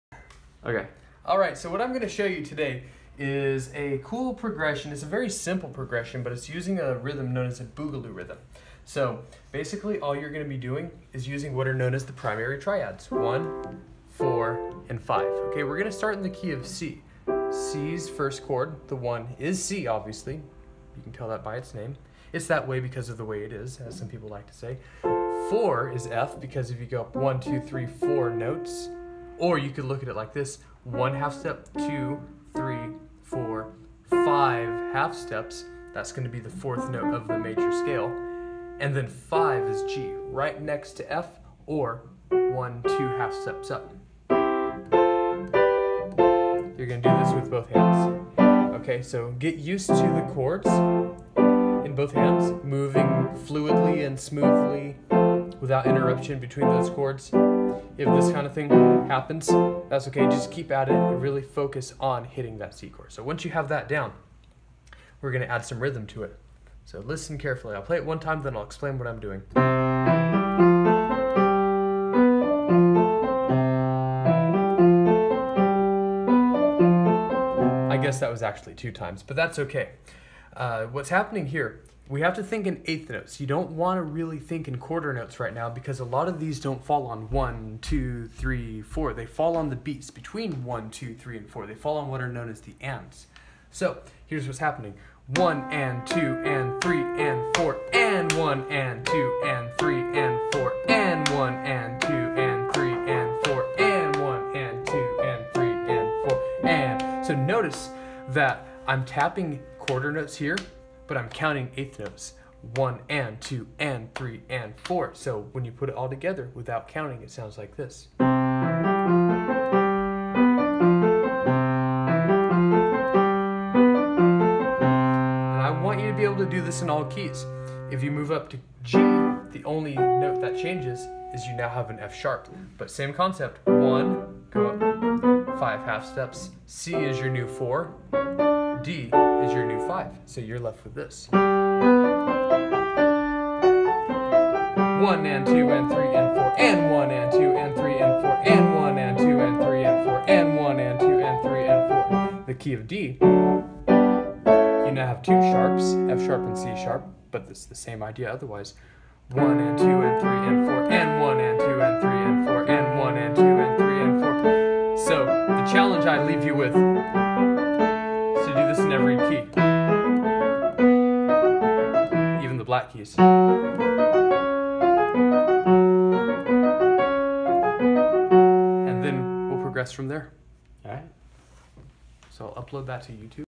Boogaloo Using Primary Triads
A fun Latin rhythm using the I IV and V chords (aka Primary Triads)